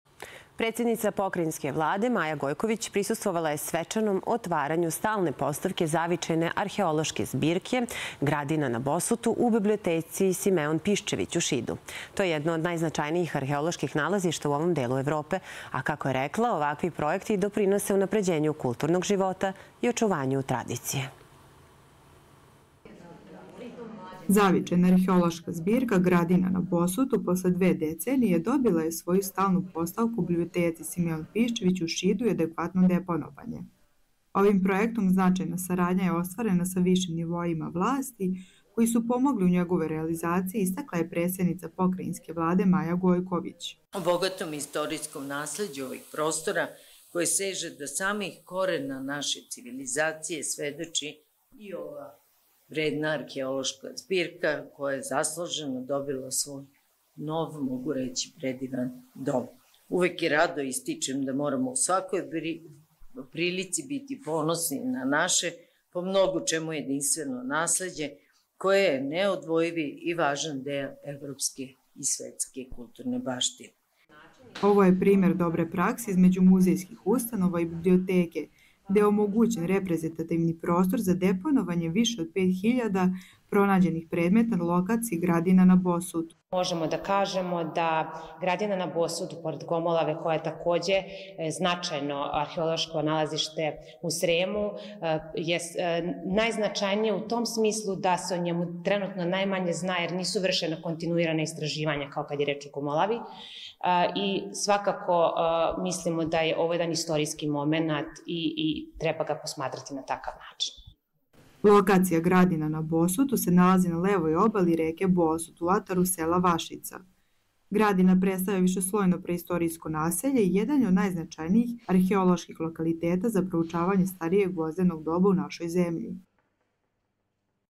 U Šidu otvorena Zavičajna arheološka zbirka "Gradina na Bosutu"
Predsednica Pokrajinske vlade Maja Gojković posetila je danas Šid, gde je sa predsednikom Opštine Zoranom Semenovićem razgovarala o aktuelnim investicijama i budućim razvojnim projektima, nakon čega je prisustvovala otvaranju Zavičajne arheološke zbirke "Gradina na Bosutu".